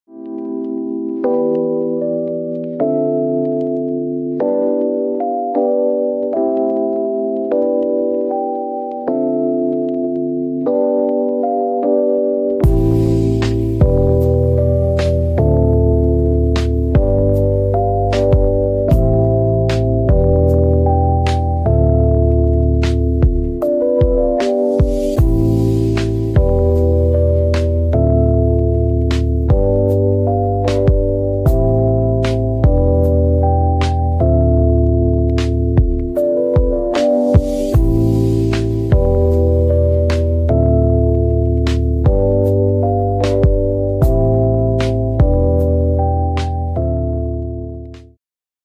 Apollonian Tree shader for MagicaVoxel
my new Apollonian Tree shader will surprise you with the variety and grace of Apollonian Gasket shapes, from elegant fairy trees to sharp spikes.